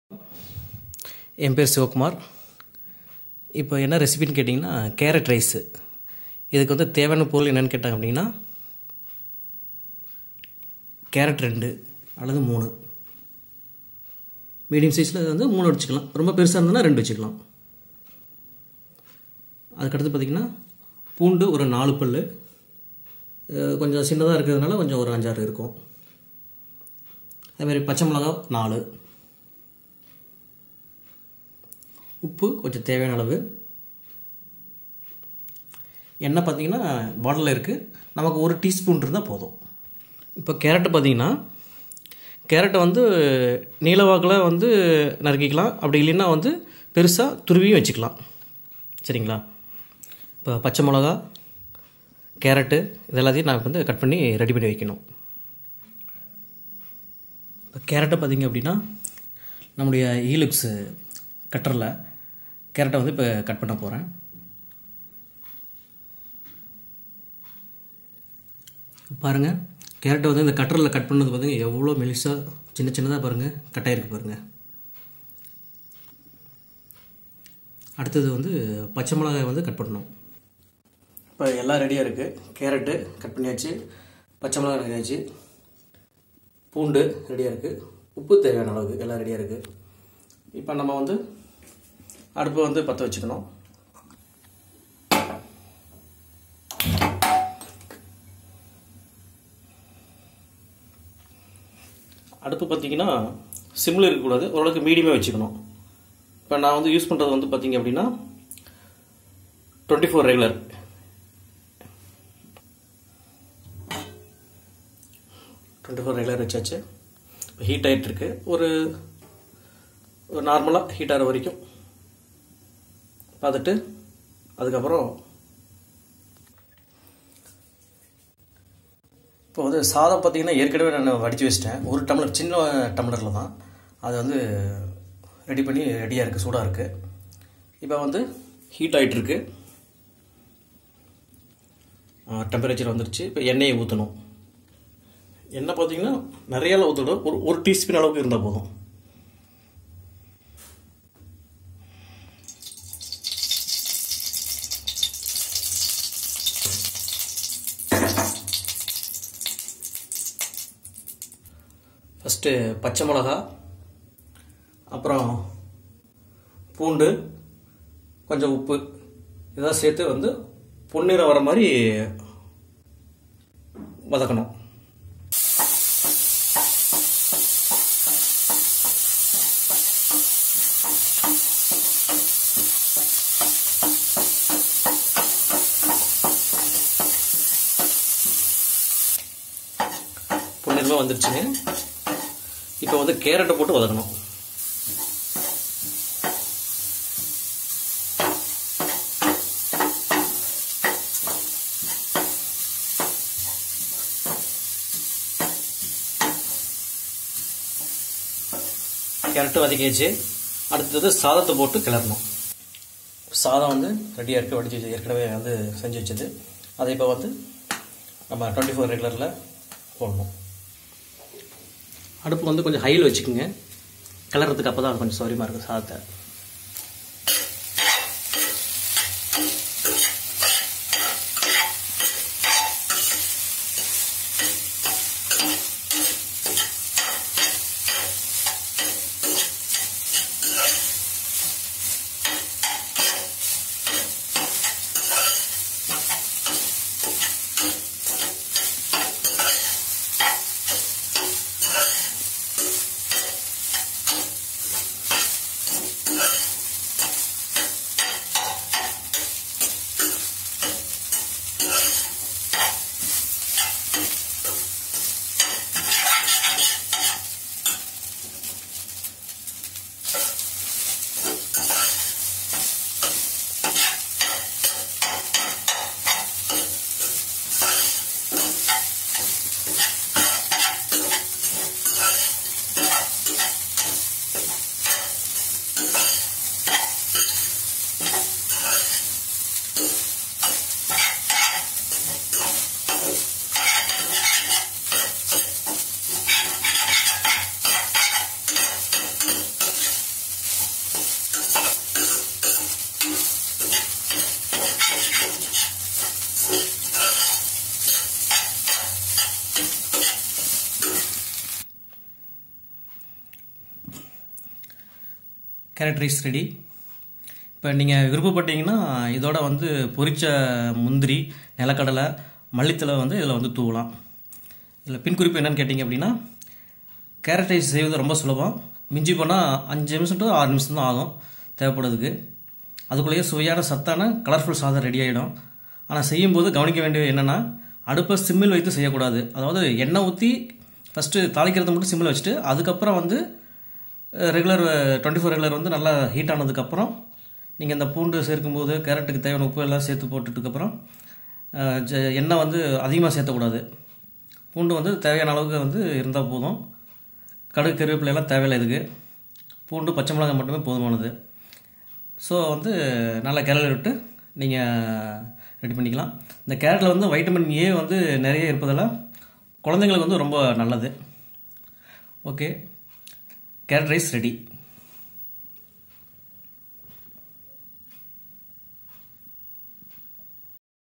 Carrot Rice Cooked in Healux Cookware| India Good Food Club
Carrot Rice Cooked by Siva Kumar from Salem

Healux Cookware